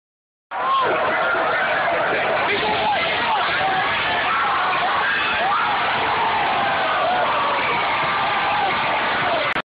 082710 Lake Zurich Bears defeat Fremd Dudek catch
Lake Zurich Bears defeat the Fremd Vikings 20 to 10 in stunning fourth quarter charge.  Dudek catch late in the game part of LZ's late charge to win it.